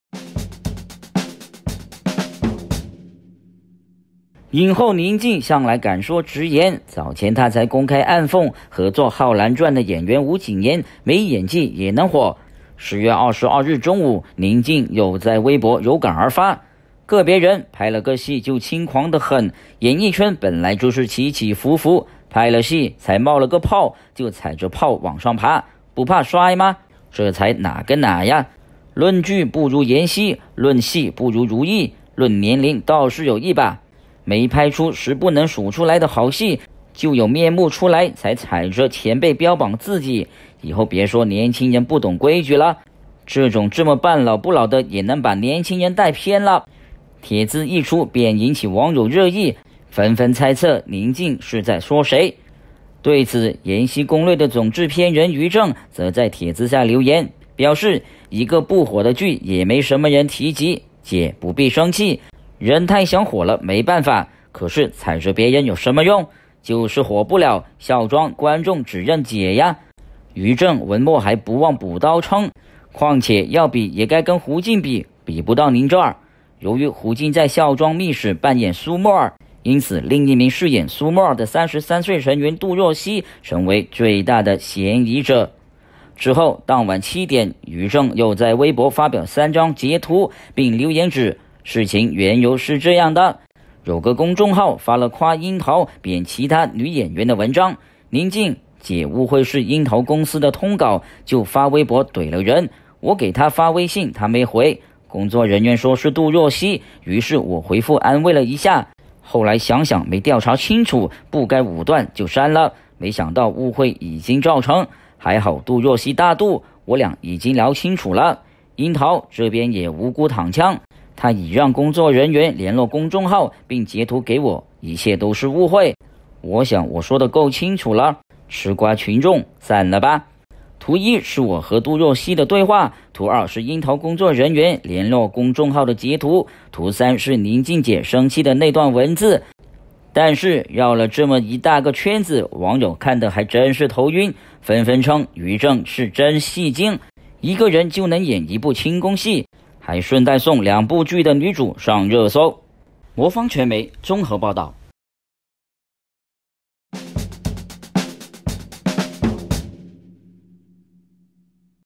2018-10-22 寧靜發文炮轟同業耍大牌 于正補刀害殷桃杜若溪躺槍
影后寧靜向來敢說直言
早前她才公開暗諷合作《皓鑭傳》的演員吳謹言
沒演技也能火
10月22日中午
又在微博有感而發
“個別人 拍了個戲就輕狂得很
演藝圈本來就是起起伏伏
拍了戲 才冒了個泡
就踩著泡往上爬
不怕摔嗎   這才哪跟哪啊
論劇不如延禧  論戲不如如懿
論年齡到是有一把
沒拍出10部能數出來的好戲
就有面目出來才踩著前輩標榜自己
以後別說年輕人不懂規矩了
這種這麼半老不老的也能把年輕人帶偏了”
帖子一出便引起網友熱議
紛紛猜測寧靜是在說誰